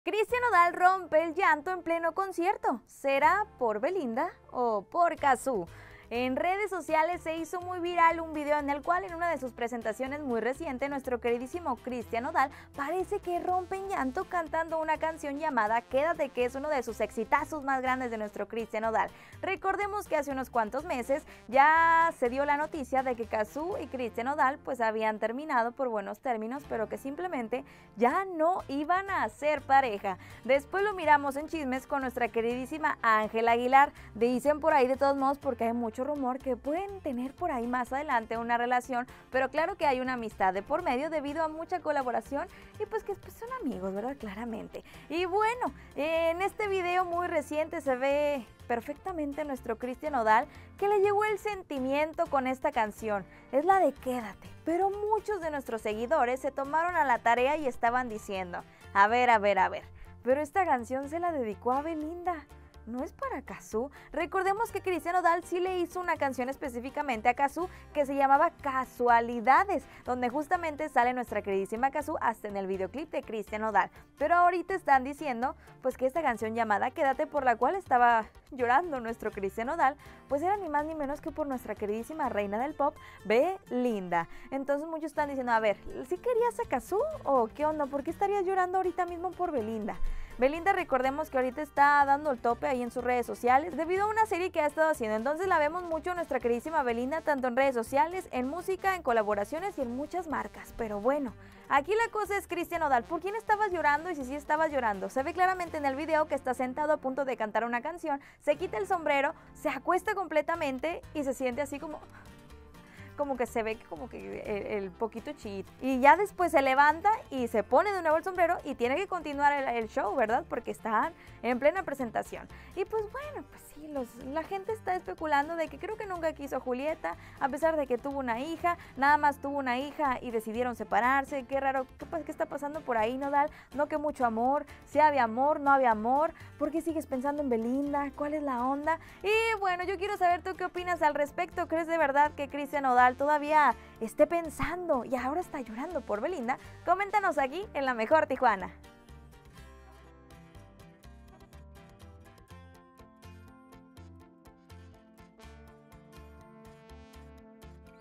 ¡Nodal rompe en llanto! ¿Es por Cazzu o por Belinda? 🤔
¡Christian Nodal rompe en llanto en pleno concierto! ¿Es por Cazzu o por Belinda? 🤔